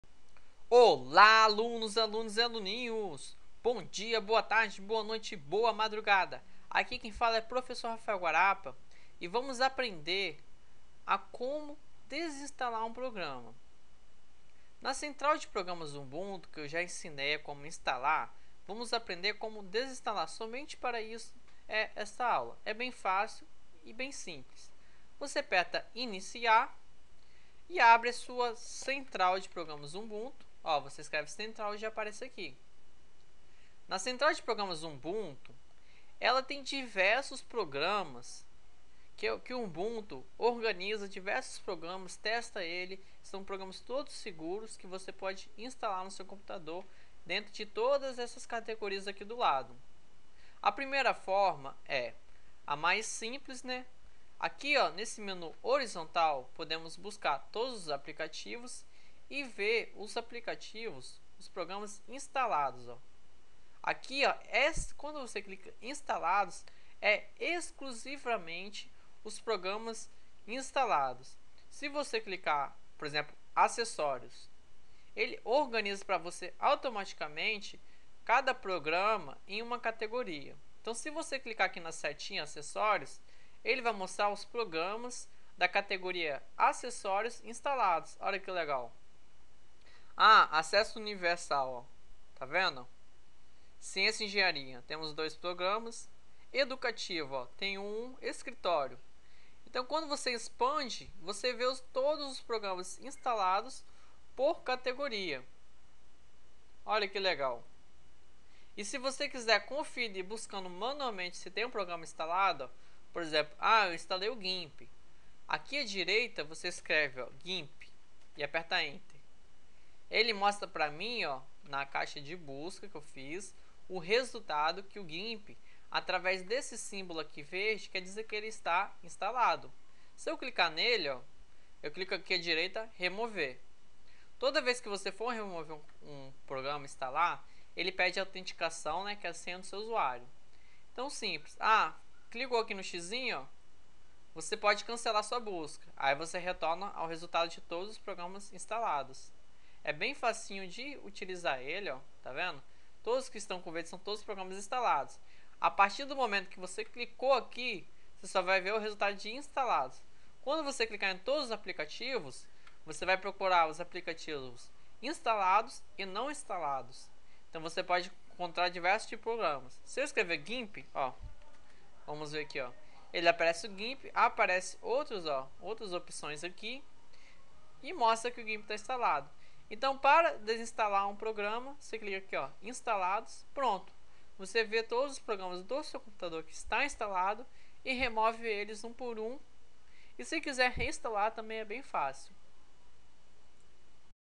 Curso de Linux Ubuntu - 07 - como Desinstalar um programa
Curso de Linux Ubuntu - aprenda a como remover, desinstalar qualquer programa ou jogo.